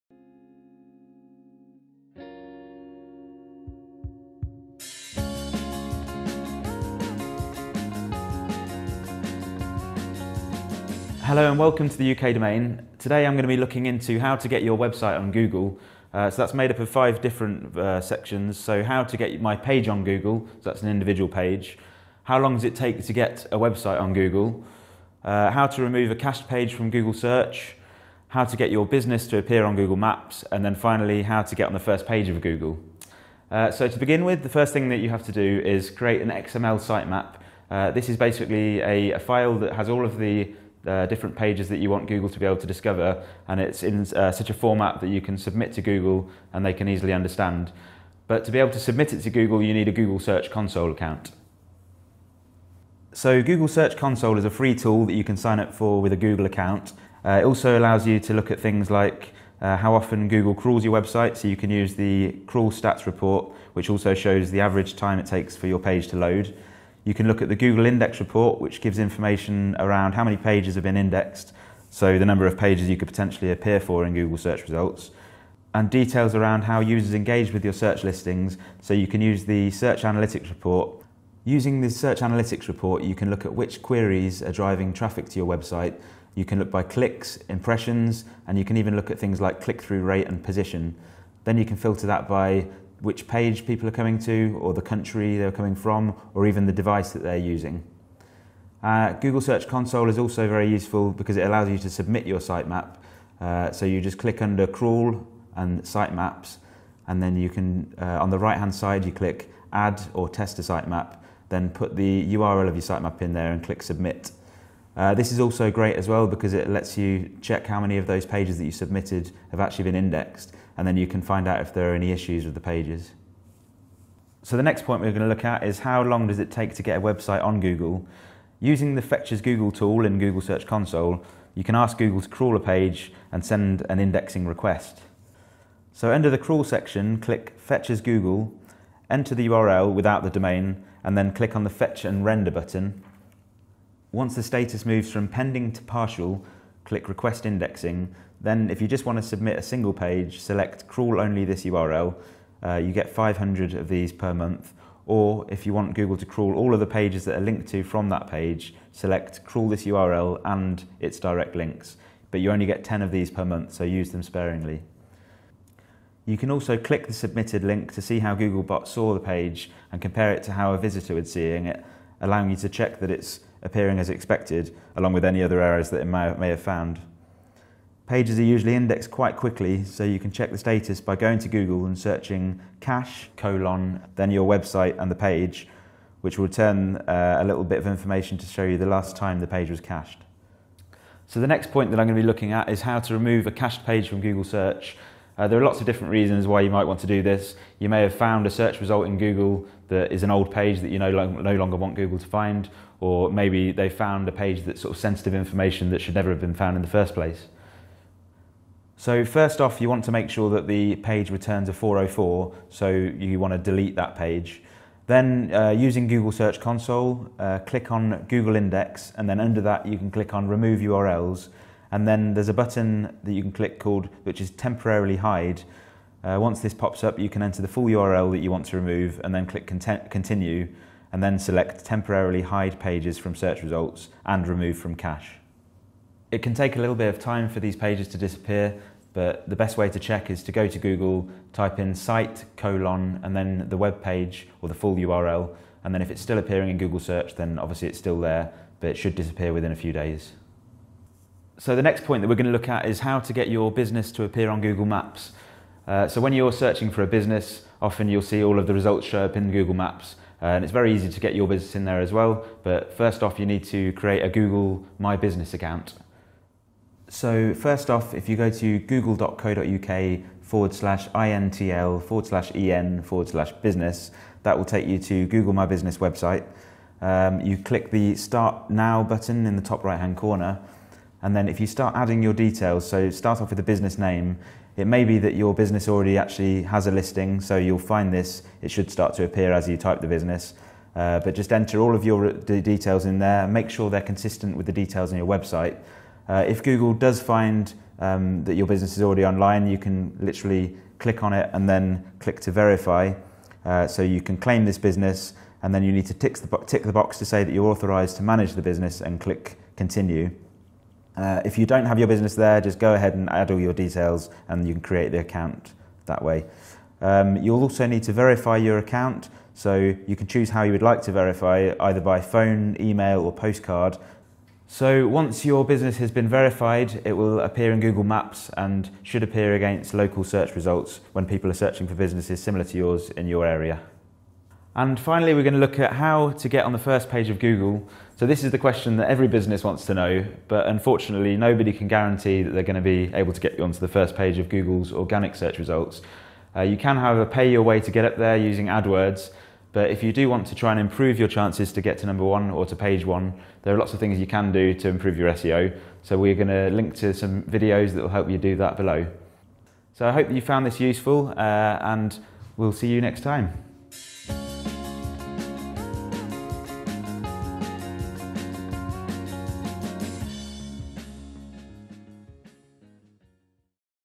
How to get your website on Google
Today Sam explains the first steps you need to follow to get your website on Google. He also covers some other common questions including how to remove a cached page from Google search and how to get your business appearing on Google maps.